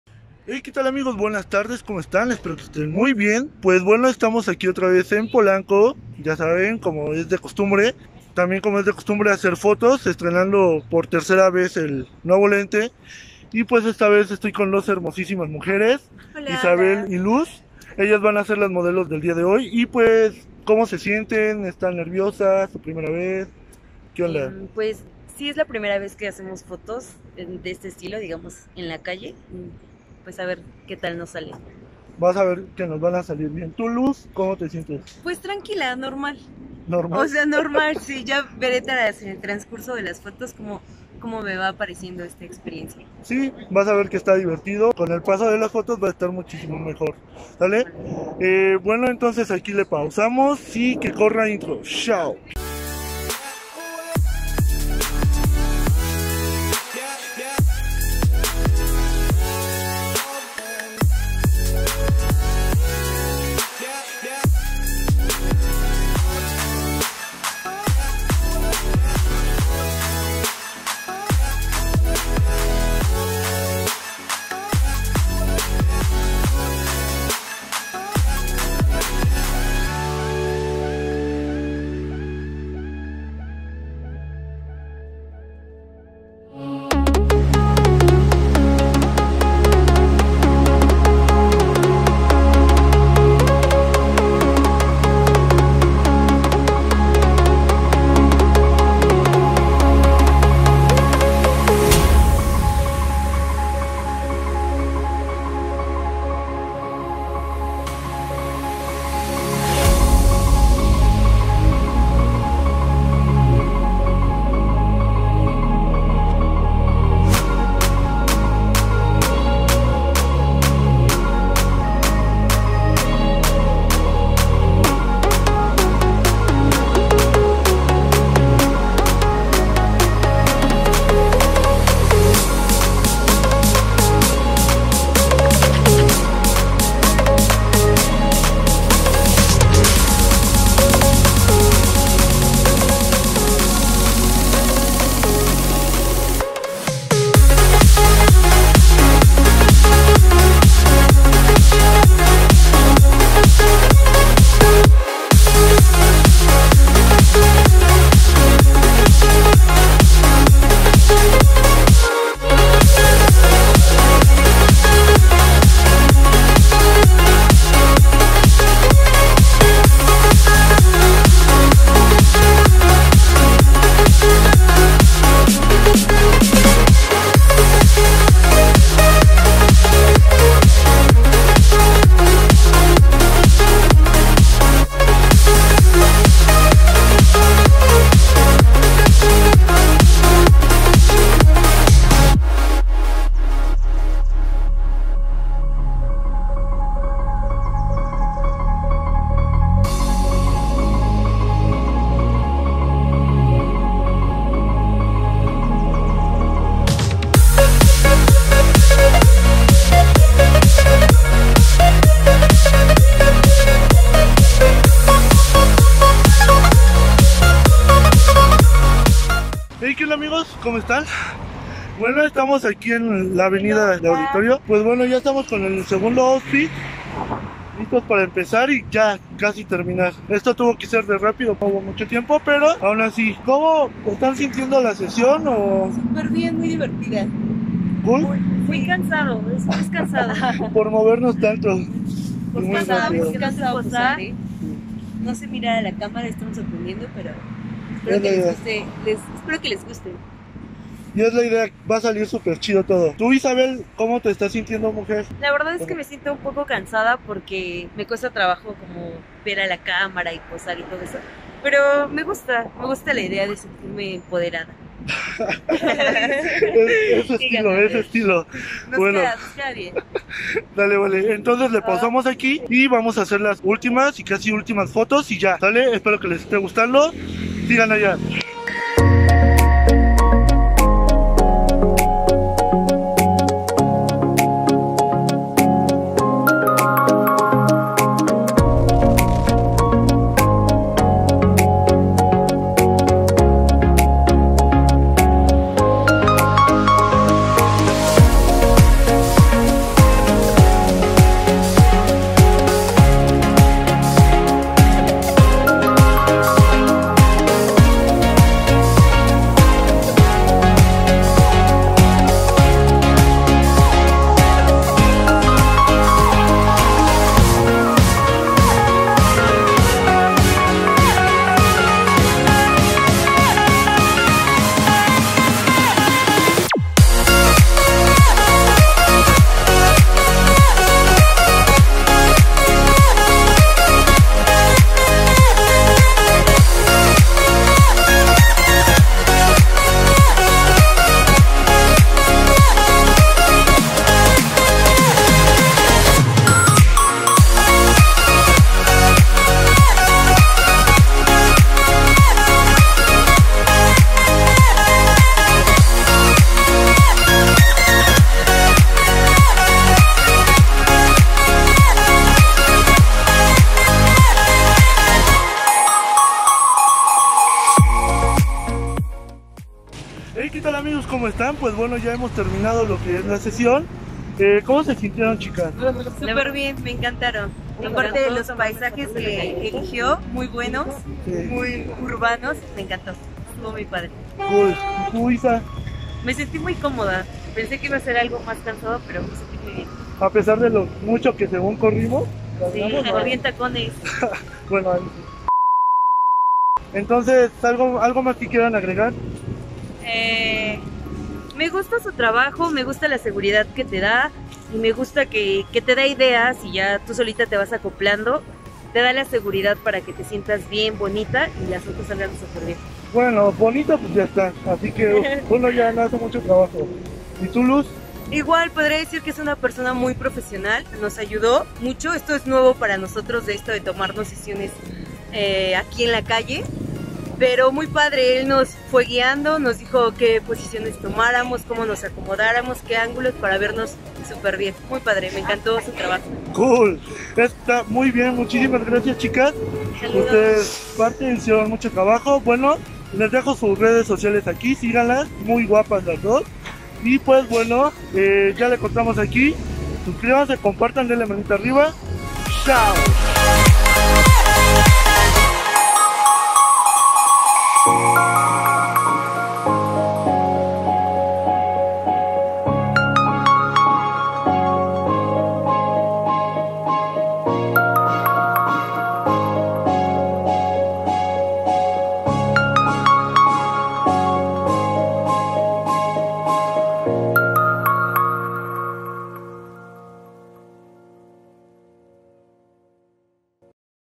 SESIÓN de FOTOS en POLANCO y AUDITORIO NACIONAL
Sinopsis: Se elaboro este shooting desde Polanco hasta Auditorio, con el fin de realizar fotos con estas dos bellísimas hermanas, que por cierto, son increíbles personas...
¡Disfruta del Blog!